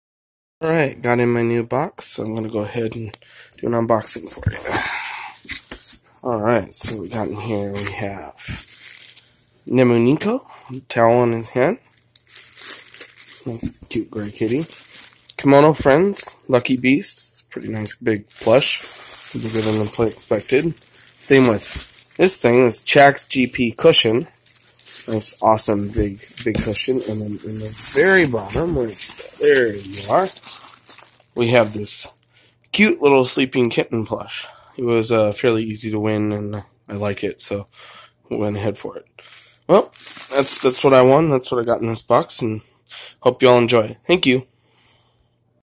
My third unboxing, whoo!
Got another fairly large box from Toreba so felt was worth unboxing it for all to see.